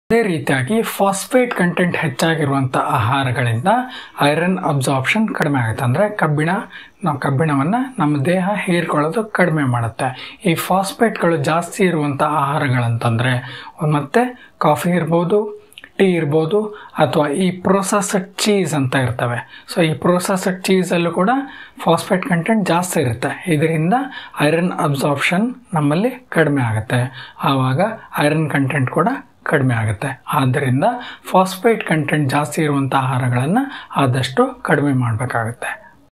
ಕಬ್ಬಿಣ ಹೀರುವಿಕೆ ಕಡಿಮೆ ಆಗೋದಕ್ಕೆಕಾರಣ Phosphateಹೆಚ್ಚಾಗಿರುವ ಆಹಾರ iron absorption shortsDescription
Iron deficiency can be sometime tackled just by avoiding some foods and by eating some foods in combination with iron rich foods and they may aid in iron absorption which is very essential for increase in serum iron content and to avoid iron deficient anemia.